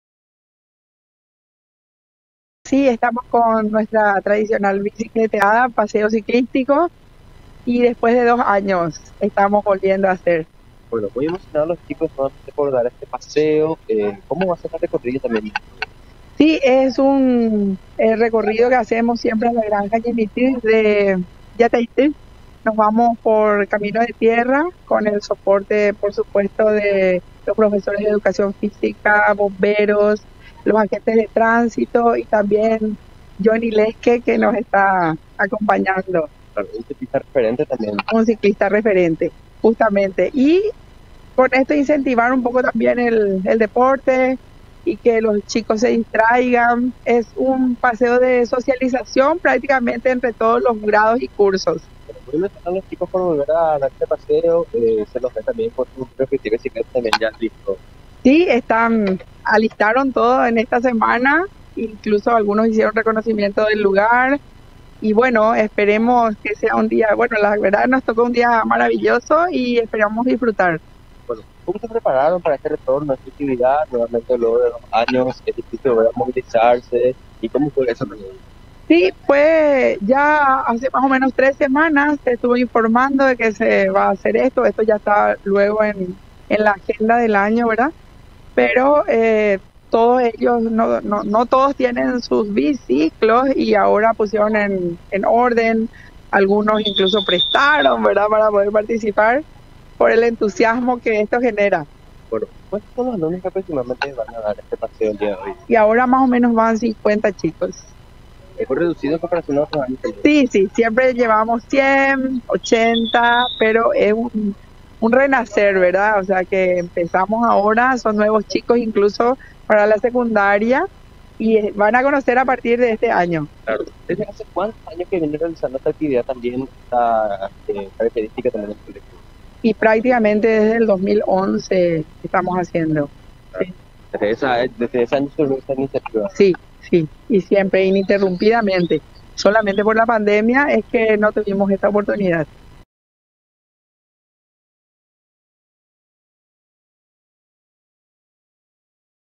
Lic. María Líz Cáceres Buscio - Vicedirectora CEI Colegio Carlos Rubén Cáceres Buscio.
Colegio Buscio retoma tradicional paseo en bici con sus alumnos.

Con el objetivo de fomentar el uso de la bicicleta e incentiva la práctica de buenos hábitos, el Centro Educativo Integral (CEI) Carlos Rubén Cáceres Buscio realiza su tradicional paseo en bici que retorna luego de 2 años. Esta recorrida inicia en la Ex Estación del Ferrocarril de la ciudad de Villarrica y culminará en un establecimiento en la ciudad de Yataity y lo realizan todos los alumnos de la institución educativa.

La Vicedirectora del Colegio Carlos Rubén Cáceres Buscio, Lic. María Líz Cáceres Buscio, comentó que el paseo en bici con los alumnos lo realizan con el instructor de ciclismo Jony Leske y en esta oportunidad están participando unos 50 alumnos de la institución.